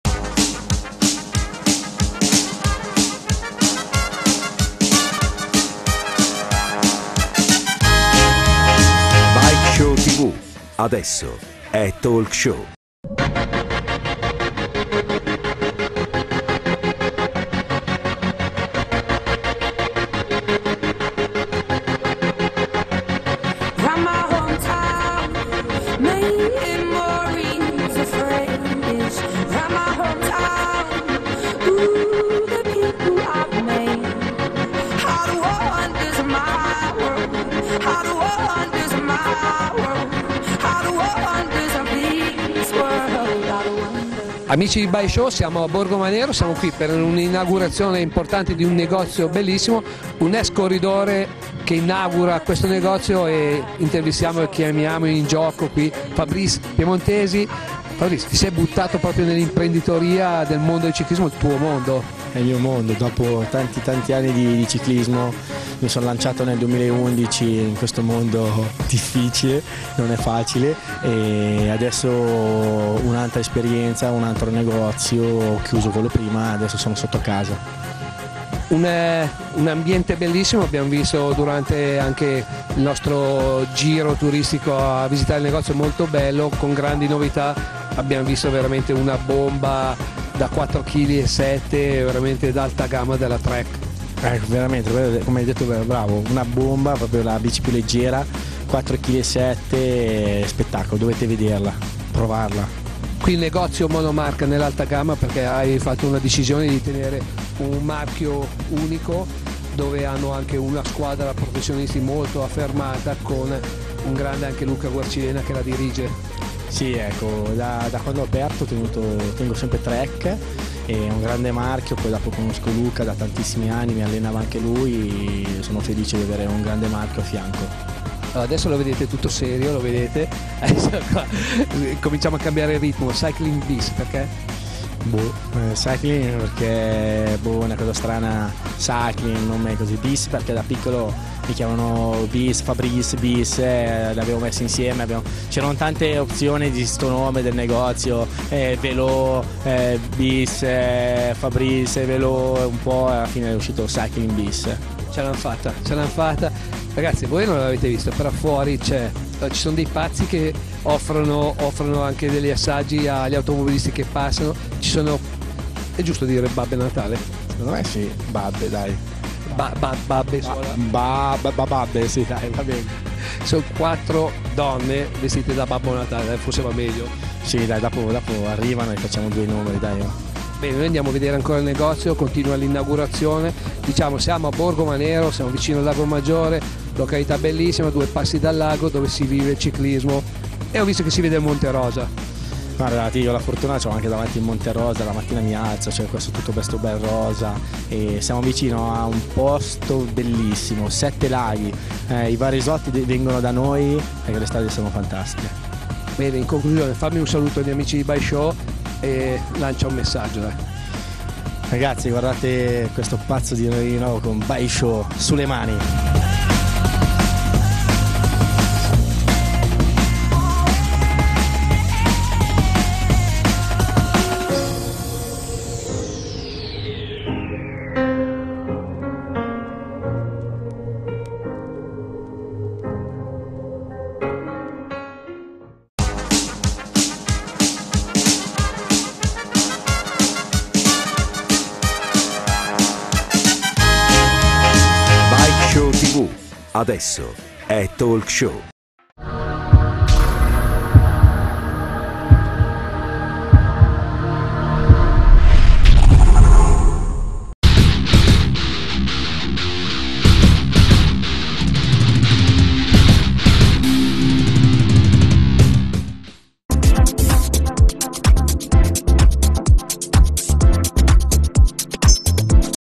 Inaugurazione Cyclingbis di Fabrice
Bike Show e Danilo Gioia alla presentazione del nuovo negozio dedicato al mondo delle bici aperto da Fabrice Piemontesi ex professionista, Cyclingbis negozio alta gamma monomarca con le specialissime Trek.